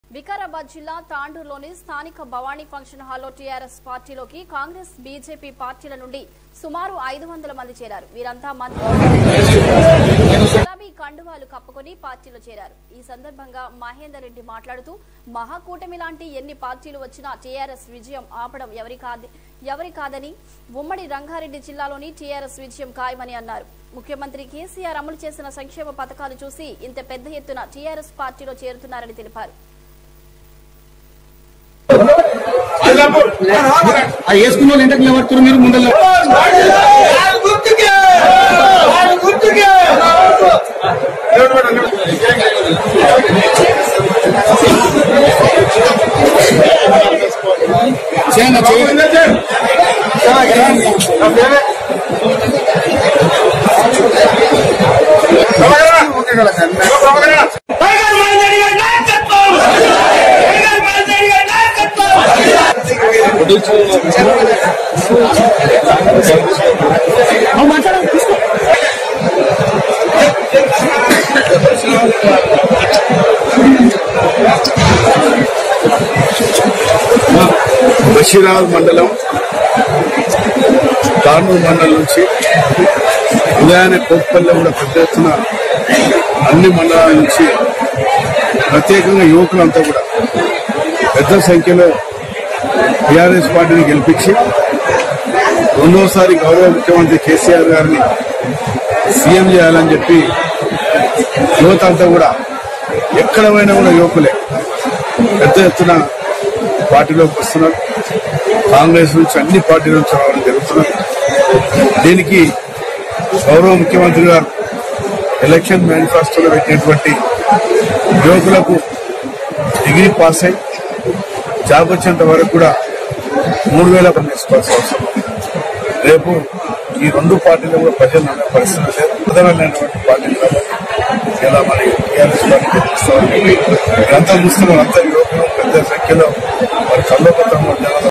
వికారాబాద్ జిల్లా తాండూర్ ..లో స్థానిక భవాని ఫంక్షన్ హాలులో టీఆరెస్ పార్టీలోకి కాంగ్రెస్
వికారాబాద్ జిల్లా తాండూర్ ..లో స్థానిక భవాని ఫంక్షన్ హాలులో టీఆరెస్ పార్టీలోకి కాంగ్రెస్, బీజేపీ పార్టీల నుండి సుమారు 500 మంది చేరారు. వీరంతా మంత్రి మహేందర్ రెడ్డి సమక్షంలో గులాబీ కండువాలు కప్పుకొని పార్టీలో చేరారు. ఈ సందర్భంగా మహేందర్ రెడ్డి మాట్లాడుతూ మహాకూటమి లాంటి ఎన్ని పార్టీలు వచ్చినా టీఆరెస్ విజయం ఆపడం ఎవరి కాదన., ఉమ్మడి రంగారెడ్డి జిల్లాలో టీఆరెస్ విజయం ఖాయమని అన్నారు. ముఖ్యమంత్రి కెసిఆర్ అమలు చేసిన సంక్షేమ, పధకాలు చూసి ఇంత పెద్ద ఎత్తున టీఆరెస్ పార్టీలో చేరుతున్నారని అన్నారు